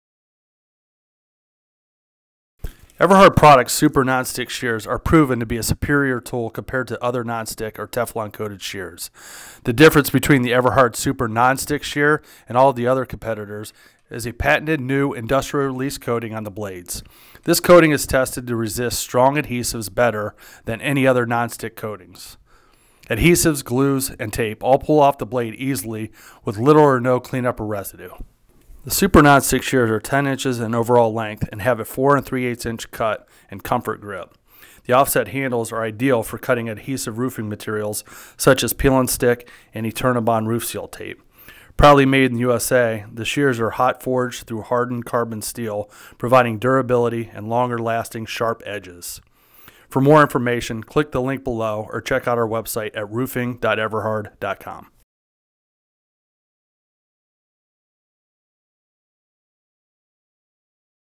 Super Non-Stick Shear
Everhard's Super Non-Stick Shears are made of heavy duty, hardened carbon steel coated with a  specialized industrial release formula. This special coating will resist strong adhesives, keeping the blades free of debris.  These shears measure 10" in length and offer a 4.75" cut length.  Available in both left (DC65925) and right (DC65915) handed version.   Made in USA.